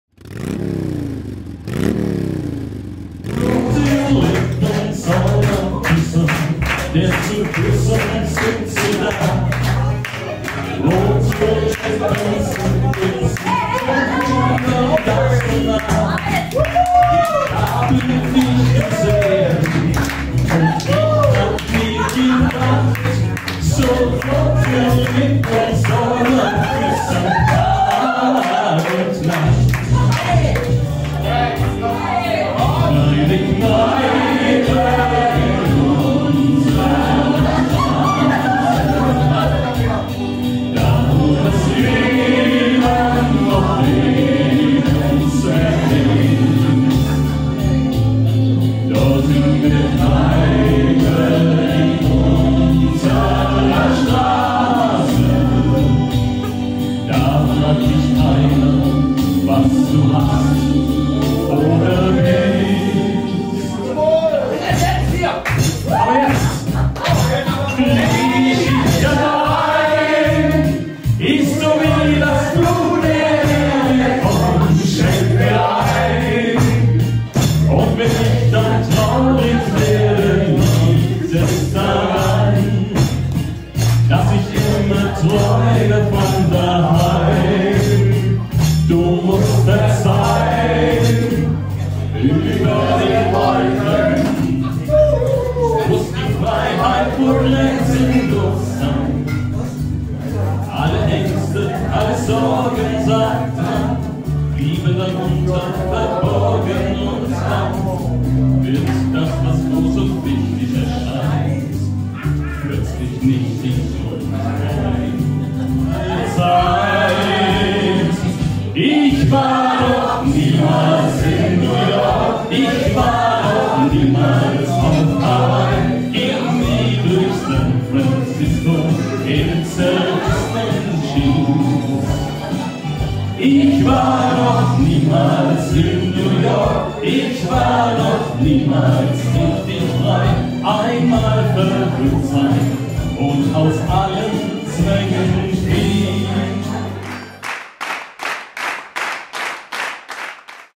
80 Jahre und kein bisschen müde
Hier seht und hört ihr eine kleine Auswahl von Titeln, die für alle Veranstaltungen geeignet sind, besonders auch für ältere Menschen, die hier in ihre Jugend zurück versetzt werden. Es ist ein Mitschnitt einer privaten Geburtstagsfeier. Mit Titeln von Udo Jürgens, Reinhard Mey, Peter Alexander u.v.a.